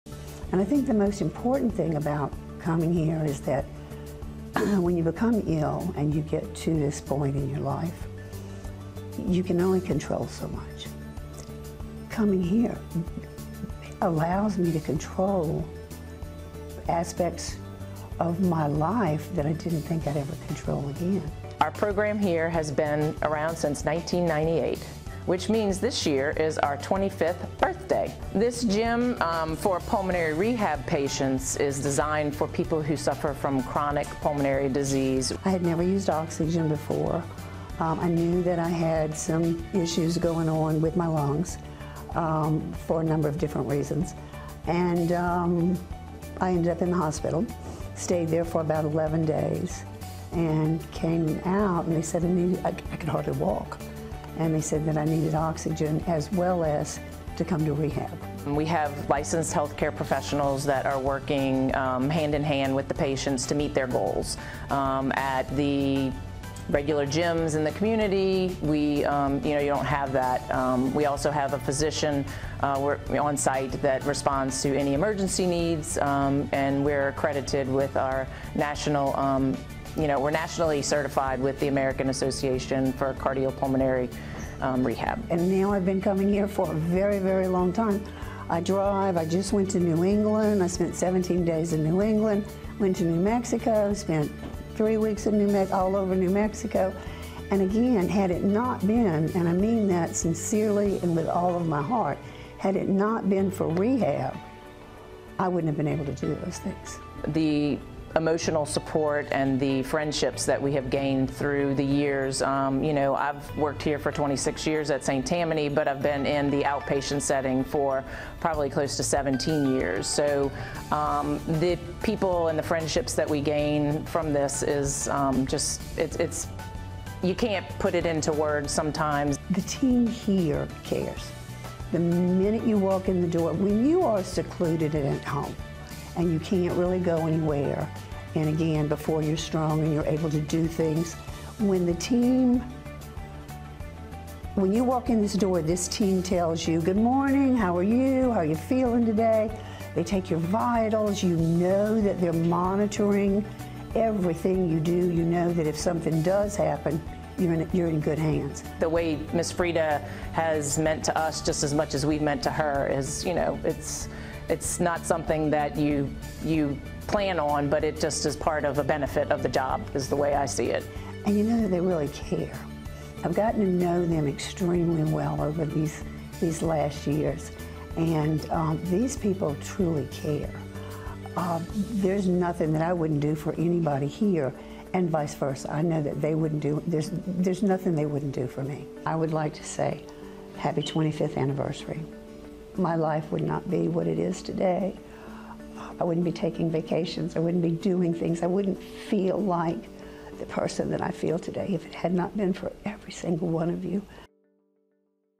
25 years of making a difference
In 1998, St. Tammany Health System launched its Cardio-Pulmonary Rehab program. In the quarter of a century since, it has improved the lives of innumerable Northshore residents. Freida Holland is one of them. Here’s her story.